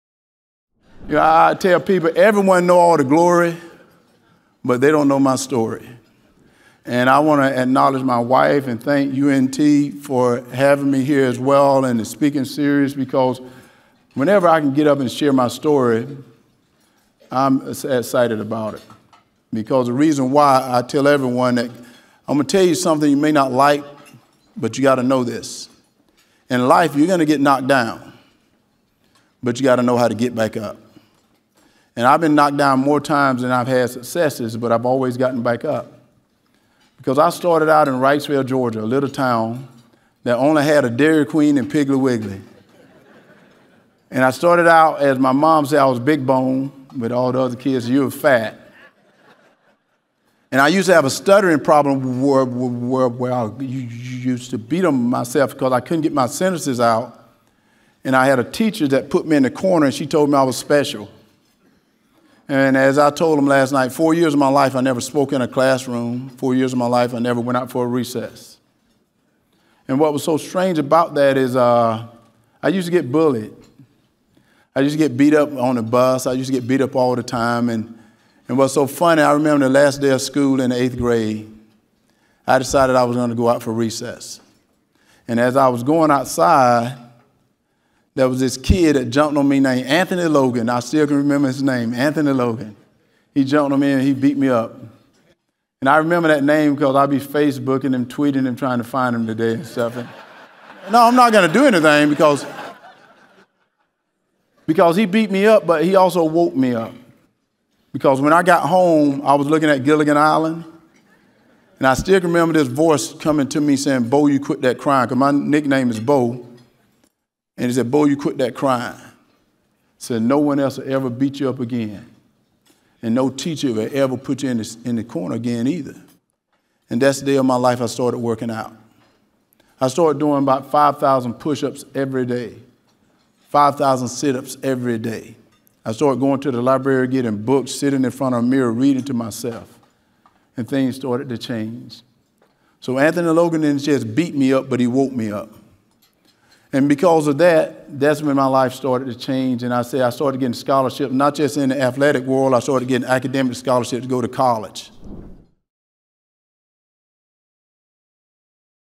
2022 UNT Kuehne Speaker Series - Herschel Walker Highlights
The UNT Kuehne Speaker Series was established in 2013 to provide a forum to engage in conversation about topics of national and global relevance. These highlights are from the Spring 2022 event featuring Herschel Walker.